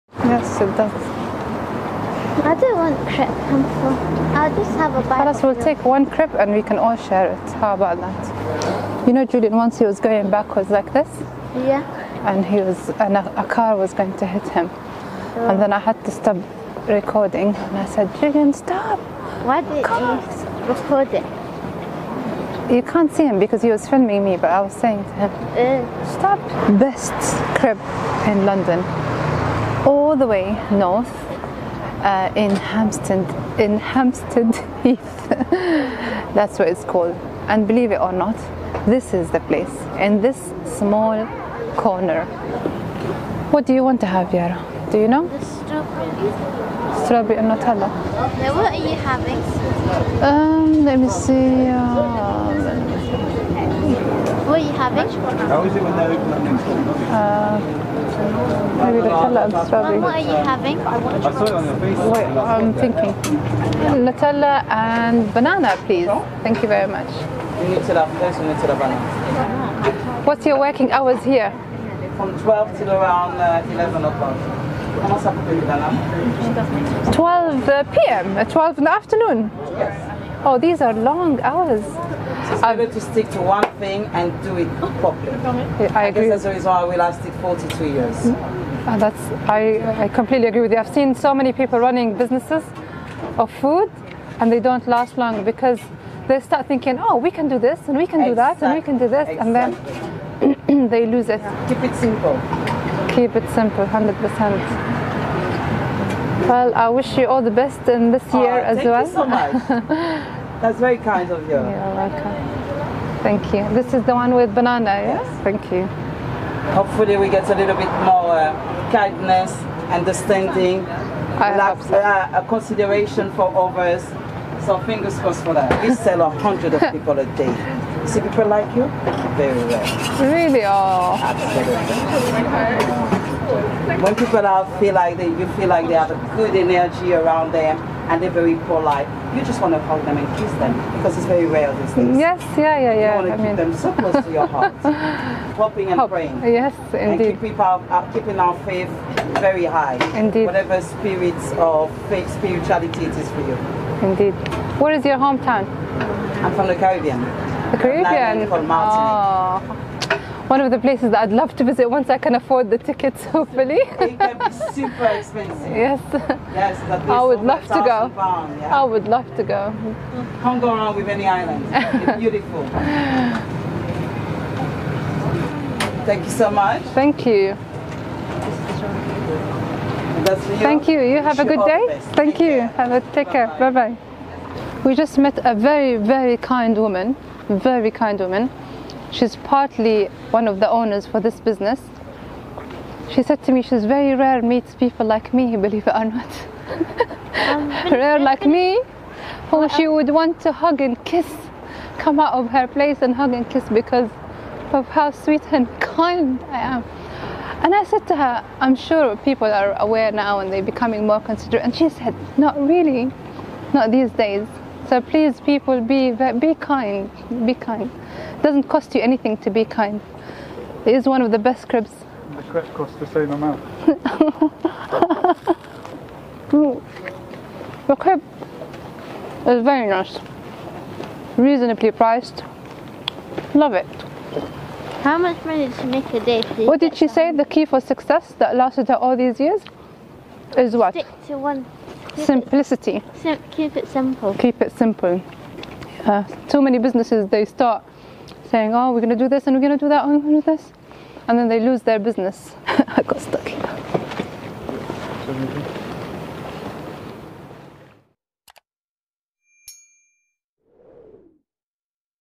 La Creperie de Hampstead
Bustling, small window-serve fixture doling out a variety of sweet and savoury crêpes.